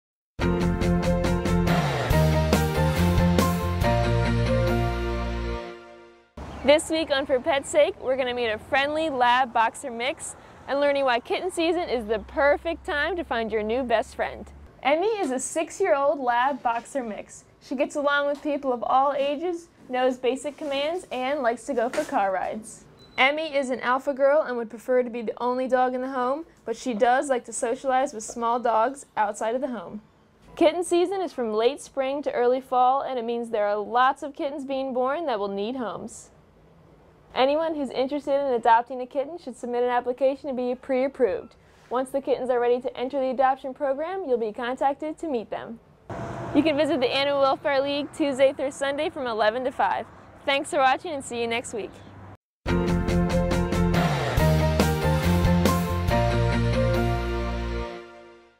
For Pets' Sake: Kitten Season!
Emmy is a 6 year old Lab/Boxer Mix that would love to have a forever home!
It is also kitten season, so there are lots of little bundles of joy that will need homes soon! Get your application pre-approved today!

And you have cable watch us on Atlantic Broadband Channel 7